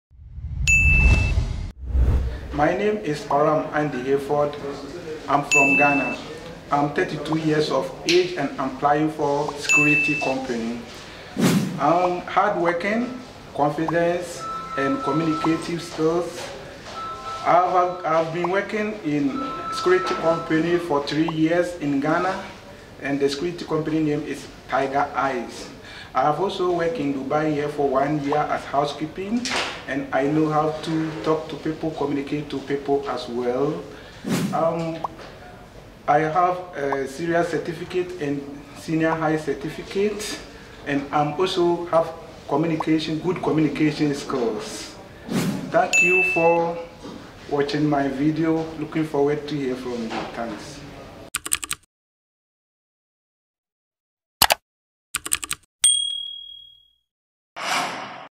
VIDEO RESUME FOR SECURITY GUARD
WorkaME
Name: Param Andy Hayford
Position: Security Guard
Country: Ghana
With experience